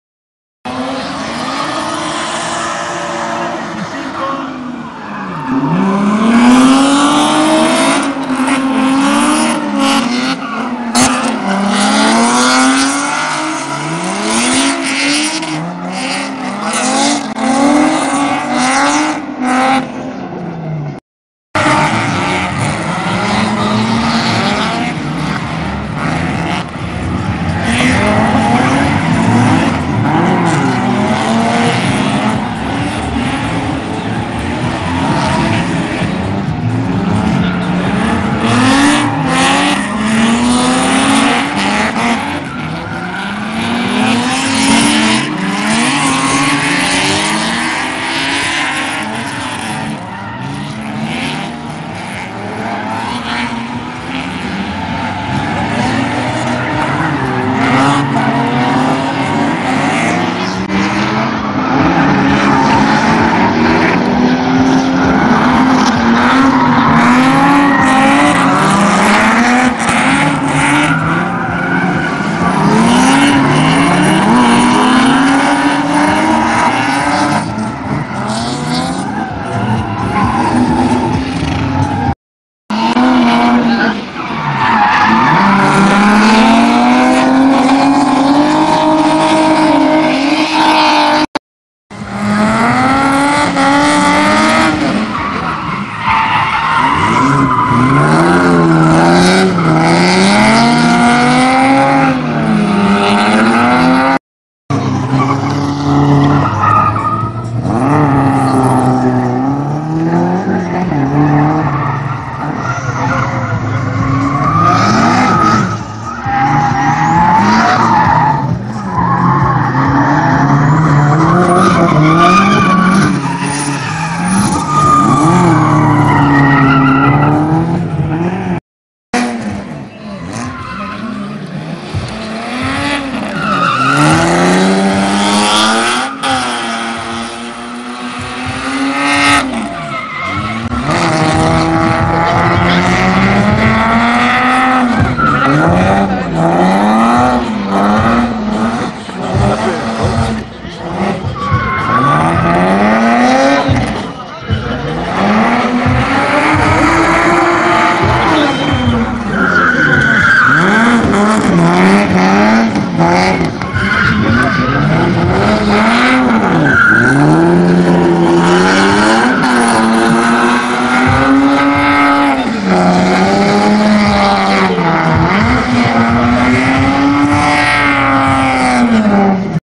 DRIFT SHOW Gran Canaria 2016
Exhibición drifting en el centro comercial El Mirador en Gran Canaria, Motor Show Festival